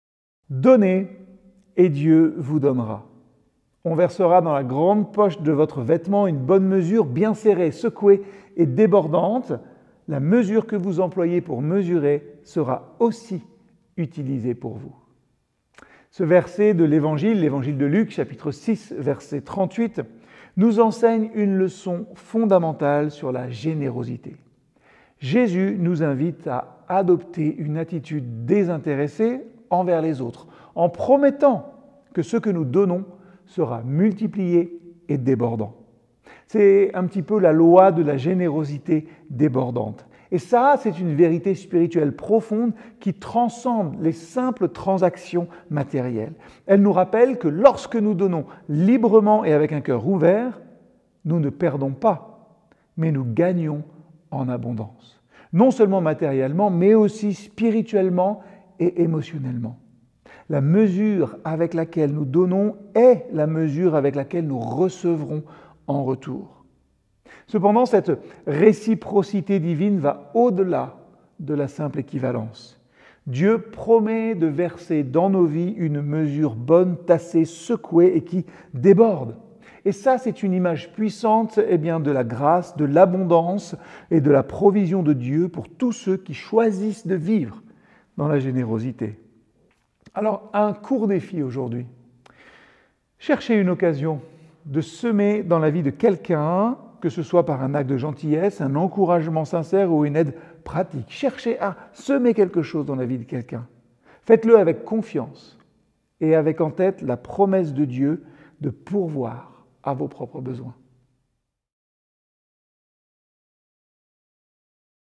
La loi de la générosité débordante - Donner Méditation 3 - Jérémie Chamard - Église M
La loi de la générosité débordante

Texte Biblique : Luc 6.38 (Version NFC)
Méditation apportée par Jérémie Chamard

Luc 6.38 (Version NFC)
38 Donnez et Dieu vous donnera : on versera dans la grande poche de votre vêtement une bonne mesure, bien serrée, secouée et débordante. La mesure que vous employez pour mesurer sera aussi utilisée pour vous. »